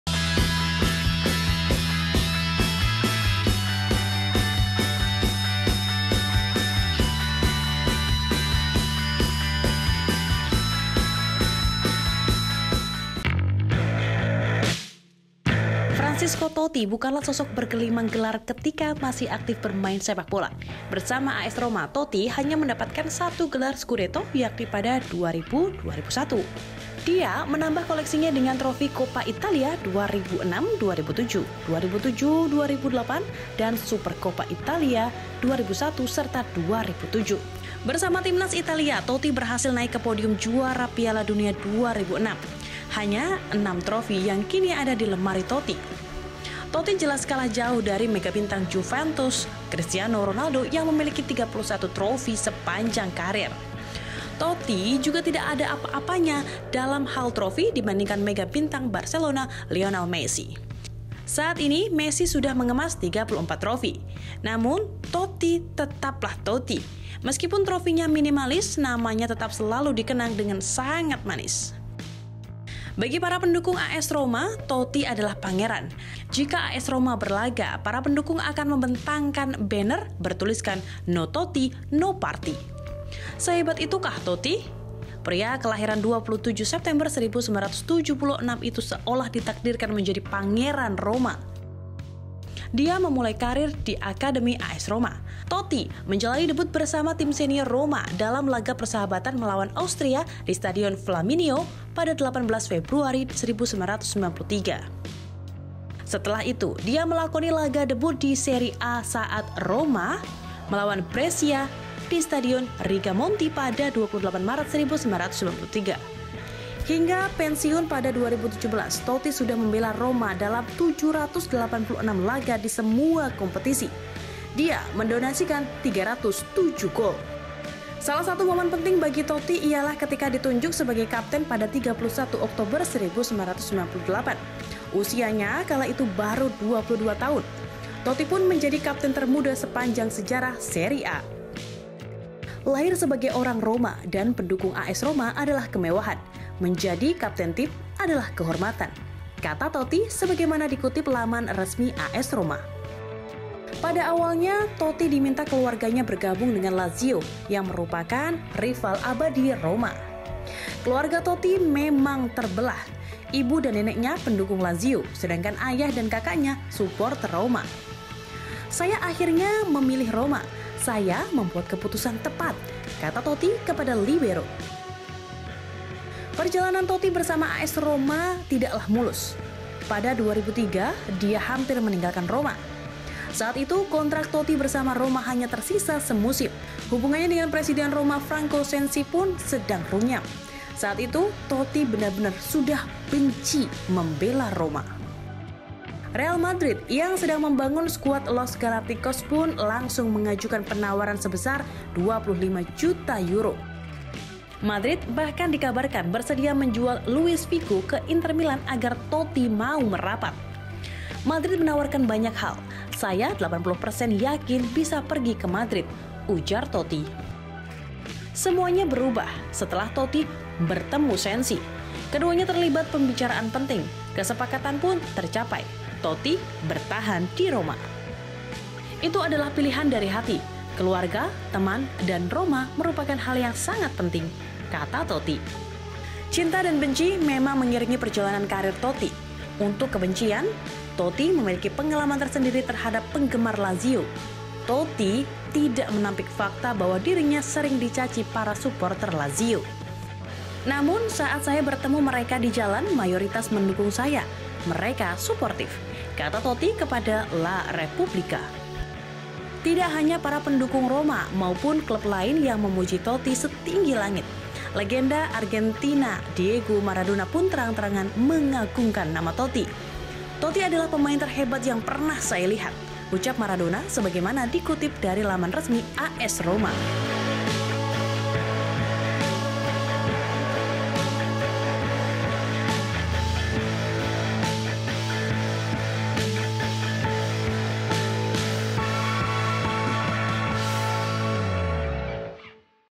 Cinta dan Benci ala Pangeran Roma Francesco Totti | G-Sport
Francesco Totti merupakan satu dari banyaknya pesepak bola dunia yang andal. Meski bukan yang terhebat, namun kehadiran Francesco Totti di AS Roma selalu dinantikan.

JANGAN LUPA INSTAL APLIKASI GENPI.CO :
APPSTORE: